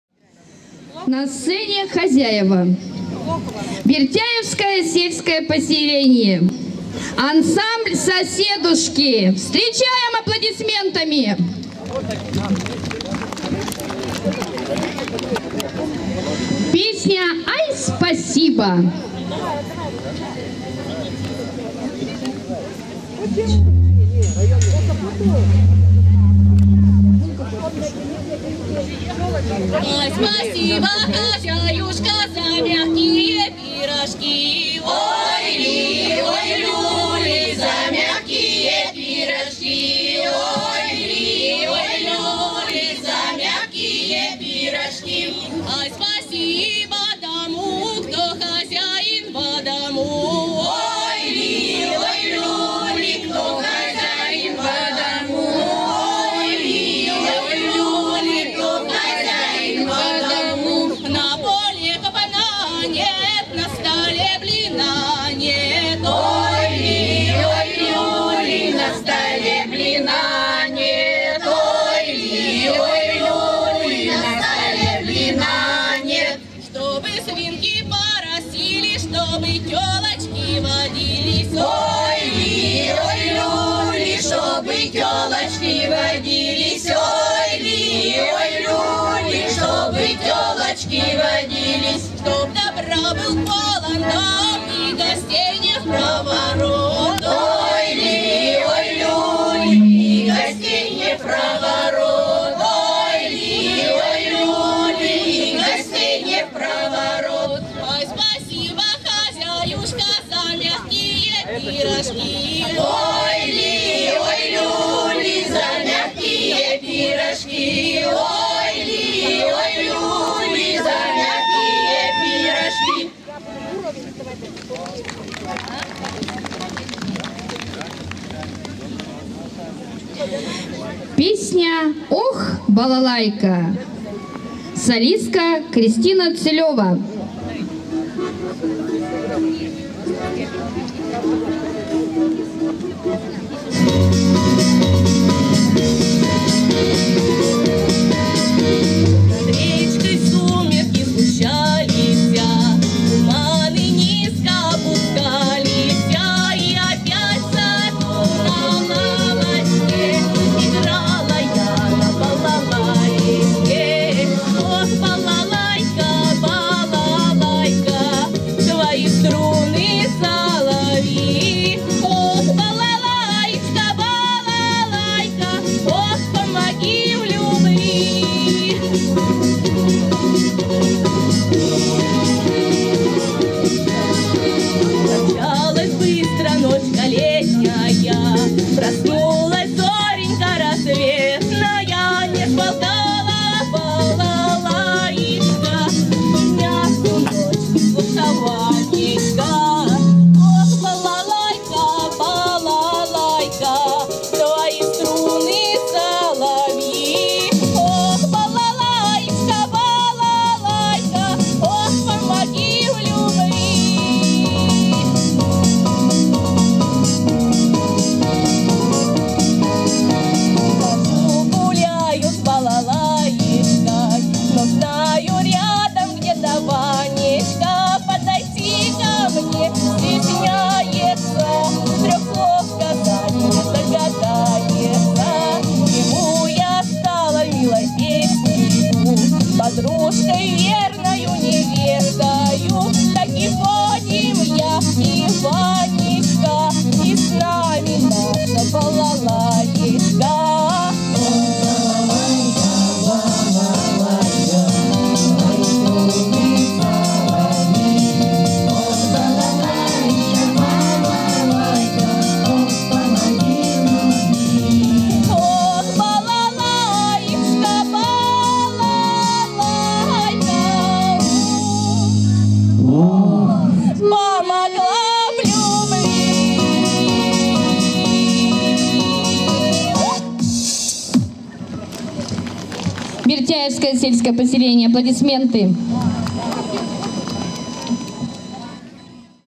Ансамбль "Соседушки" - 2018 год Иванов день (фрагмент концерта)
Областной праздник русской культуры "Иванов день" - 7.07.18. Выступление ансамбля "Соседушки" - Котельничский район ВЦРК видео 2018г.